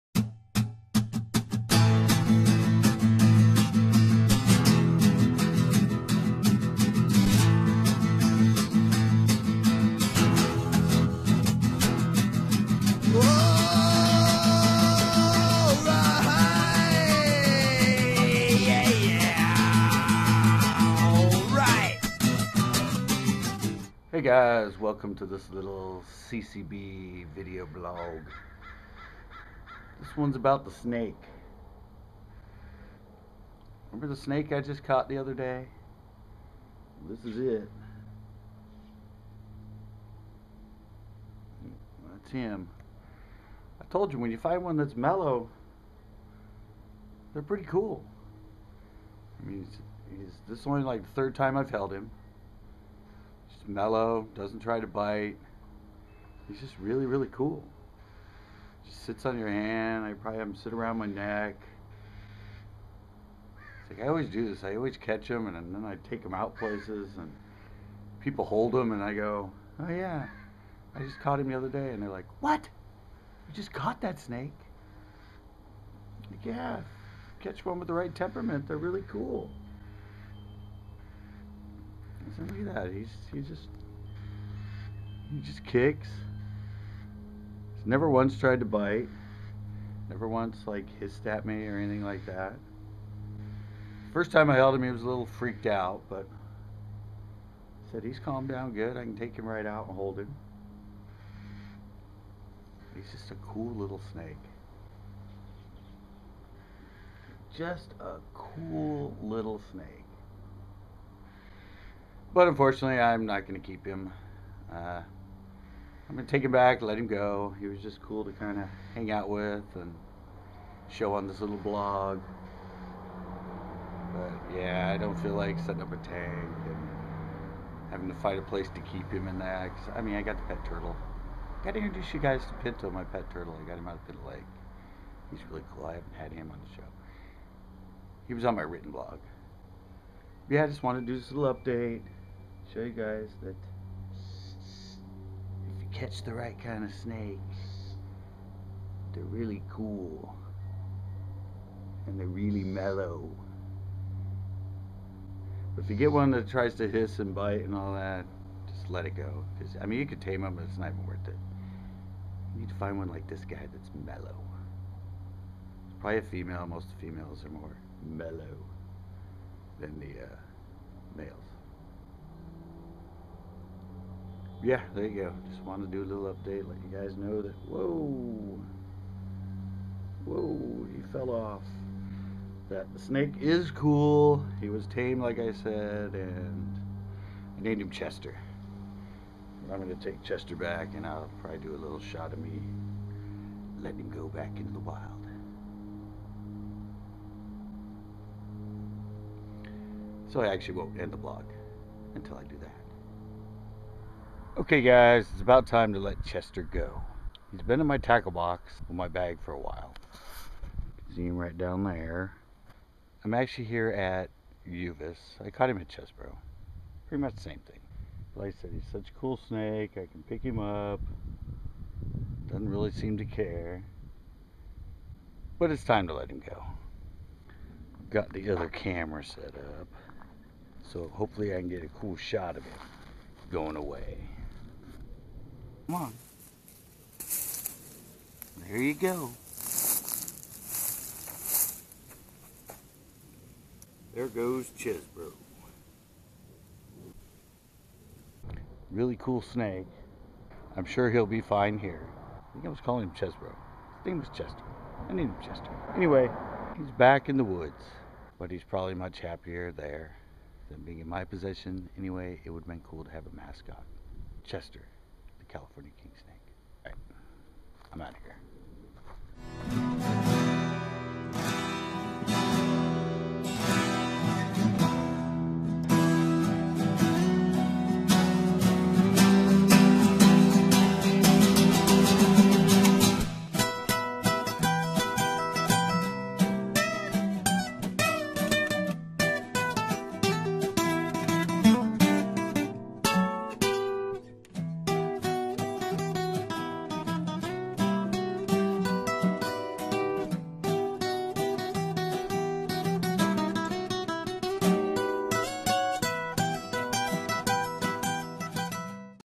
Update on CHESTER THE KING SNAKE.
Central coast bass video blog, adventures, interviews, exploration, or just whats on our mind. Chester is one of the coolest snakes I've caught. Here is an update.
To view all our shows . Enjoy.